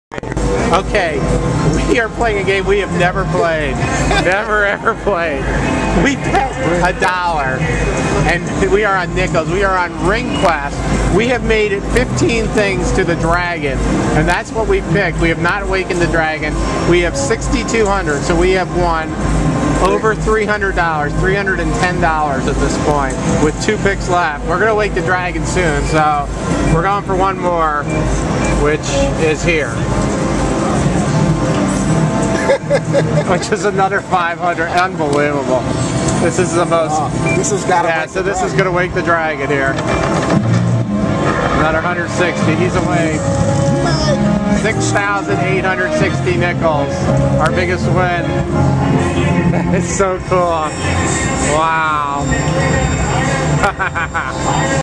Huge Win! -- Ring Quest -- Soaring Eagle -- 06/05/10
The night continues to get better.  Scott and I playing nickels have bet $1.00 and gotten to the bonus round.  You only see the end of this video -- we got 4 rings to trigger the bonus and traveled 15 steps to get to the Dragon Treasure.